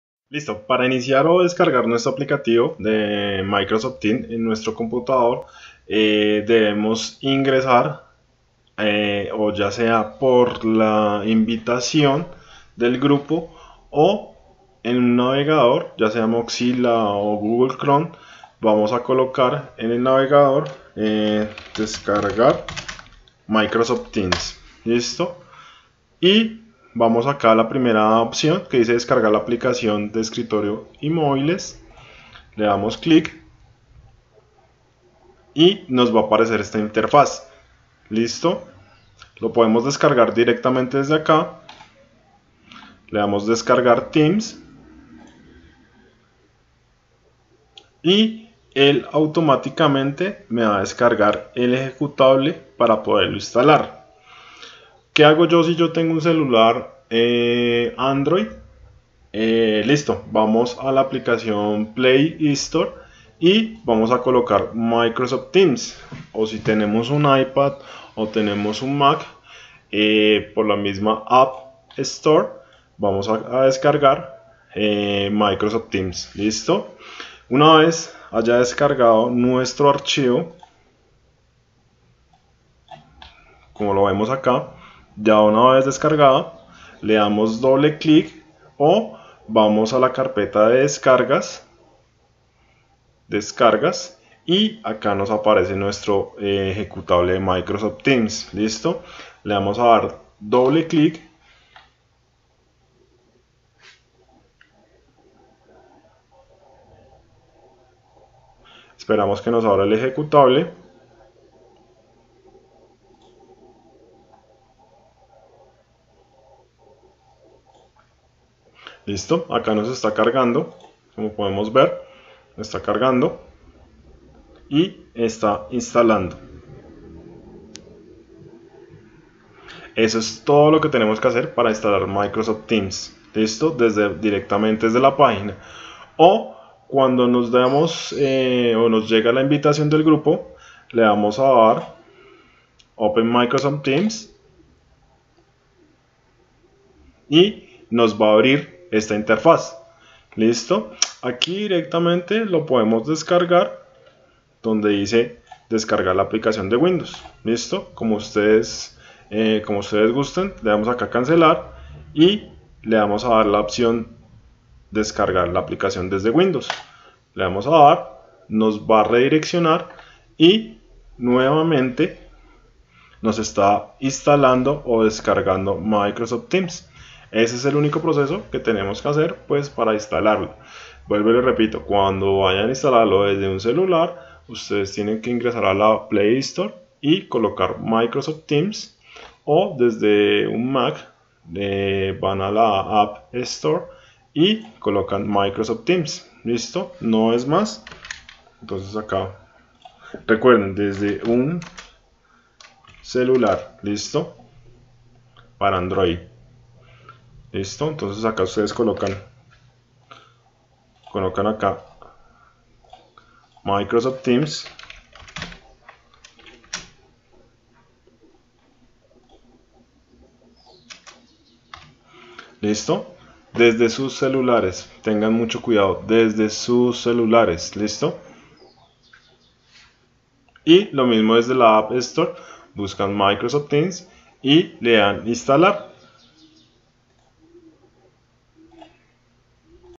TUTORIAL DESCARGAR E INSTALAR TEAMS
Tutorial en el que se explica el proceso para descargar la aplicación Microsoft Teams en computadores y dispositivos móviles.
Elaborado por Sergio Morris, encargado de Sistemas para los cuatro colegios distritales administrados por los Hermanos Maristas en Bosa.